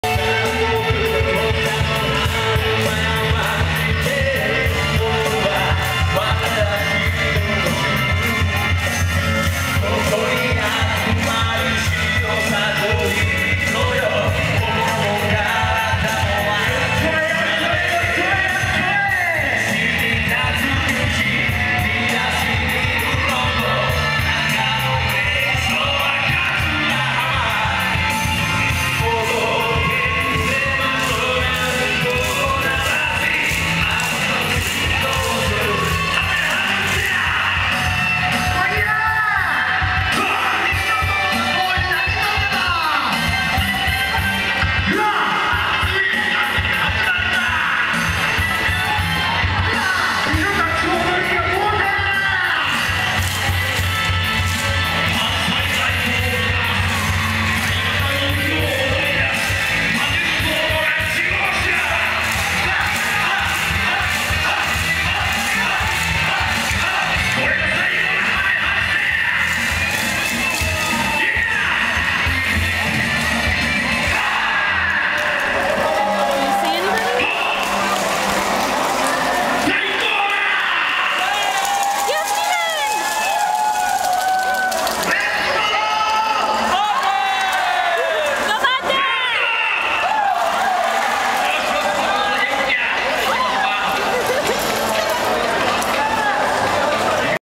Yosakoi Kochi 2008 - ハルノ四郷人
Filmed on location in the Obiyamachi area  of Kochi City, Kochi, Japan.